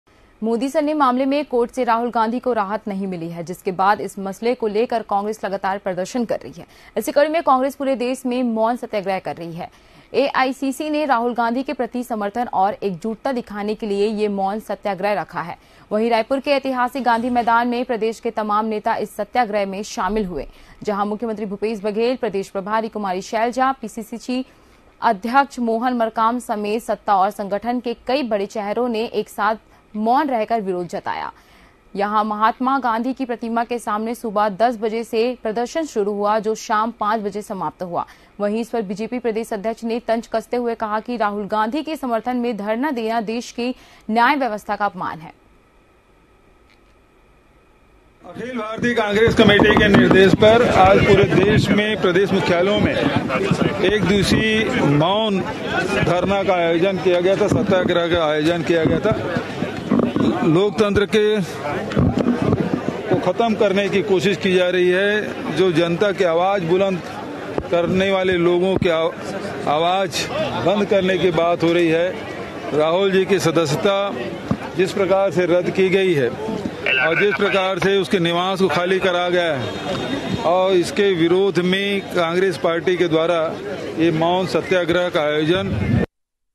कांग्रेस के मौन सत्याग्रह पर बोले C M Bhupesh Baghel | TV24 MP/CG NEWS
Video Tilte :-कांग्रेस के मौन सत्याग्रह पर बोले C M Bhupesh Baghel | TV24 MP/CG NEWS